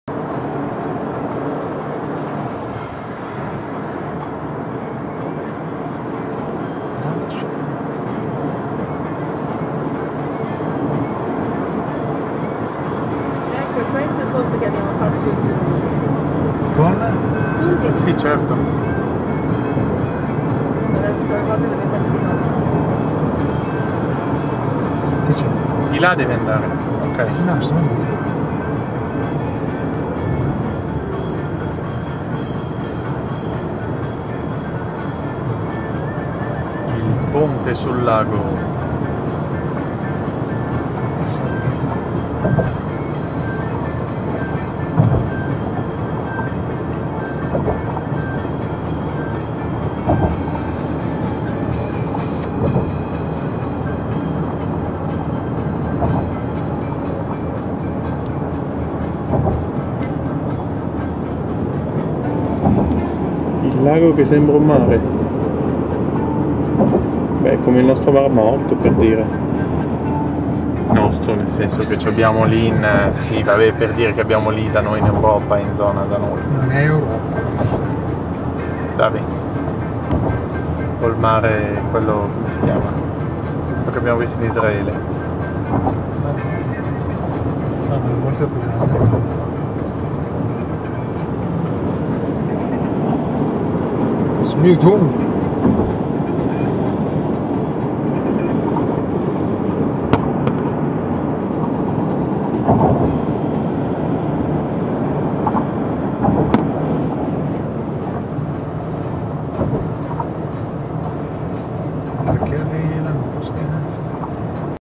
Sulla strada QEW per Guelph
La strada che dalle cascate del Niagara porta a Guelph. Il ponte vicino ad Hamilton